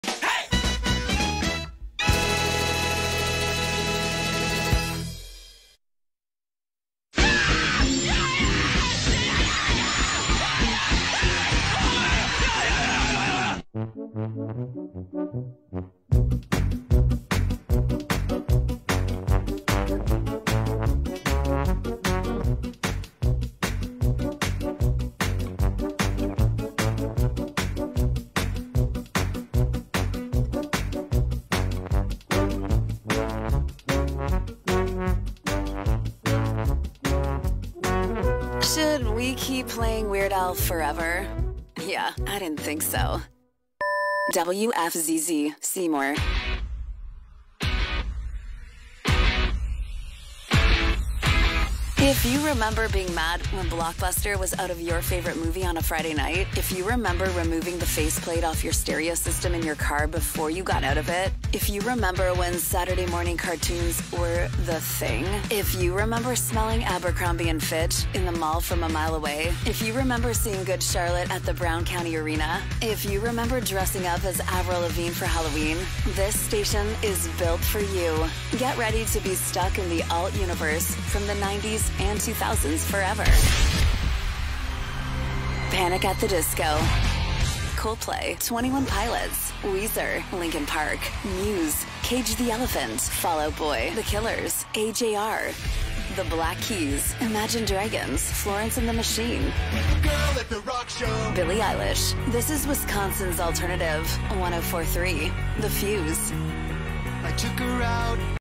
Format Change: Alternative '104.3 The Fuse' Debuts on WFZZ/Seymour, Wisconsin - January 3, 2023
After a week of stunting with all-Weird Al music, the former WKZG officially flipped to alternative as WFZZ, '104.3 The Fuse', bringing the format back to Northeast Wisconsin for the first time since 2017. A longer aircheck featuring the last 85ish minutes of 'Weird Al Radio' (as well as other parts of the stunt I mentioned previously) and the first two or so hours of 'The Fuse' will be edited/posted upon my return from Texas.